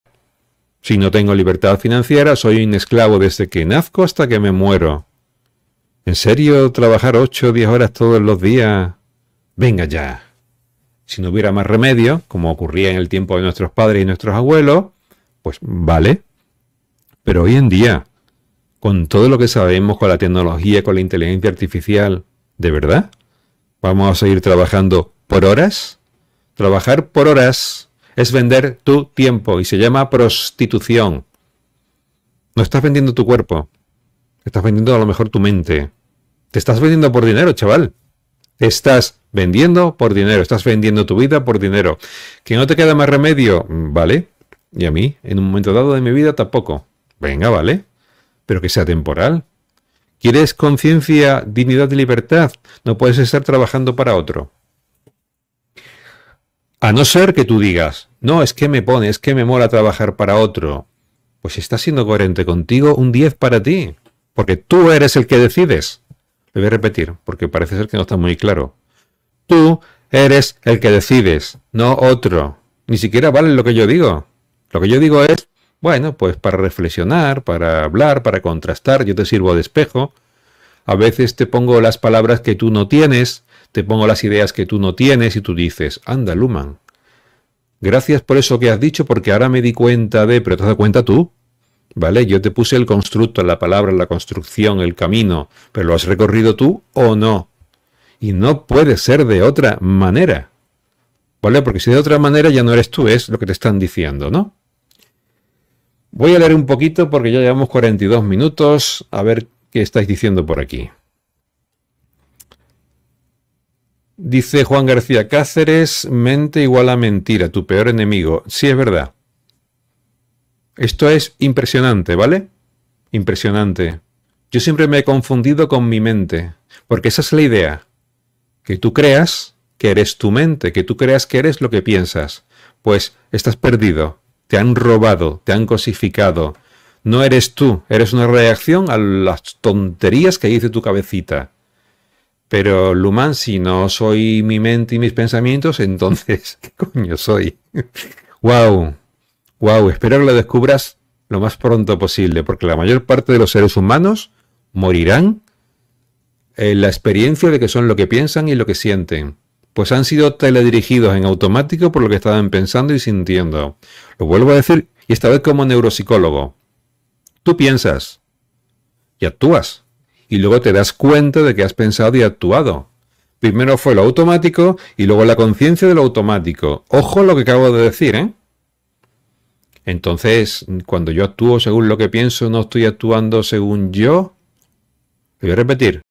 ¡Despierta! La Verdad sobre la Libertad Financiera
En este video, descubrirás la verdad sobre la libertad financiera. ¡Despierta y aprende cómo alcanzar la libertad financiera para vivir la vida que deseas!